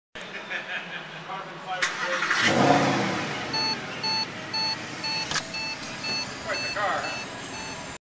Turning the SLS AMG on
Take a look as we turn the engine of the SLS AMG on